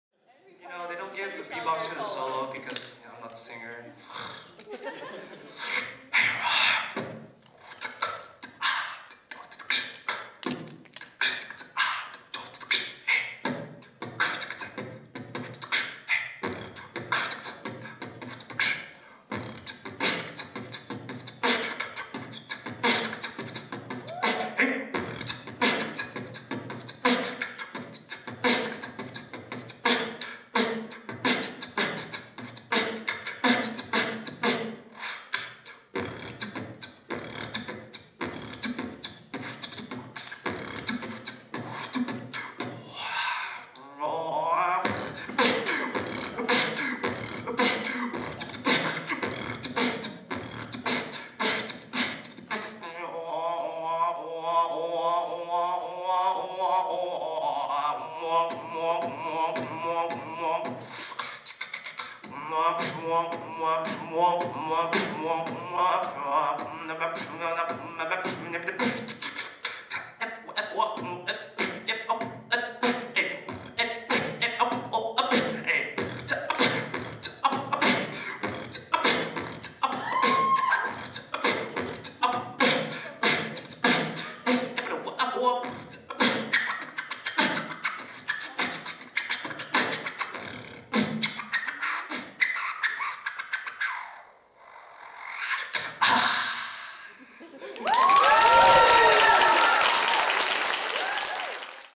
NOVIS Spring 2018 - Darwin's Solo
Darwin's (beatboxer) solo during intermission.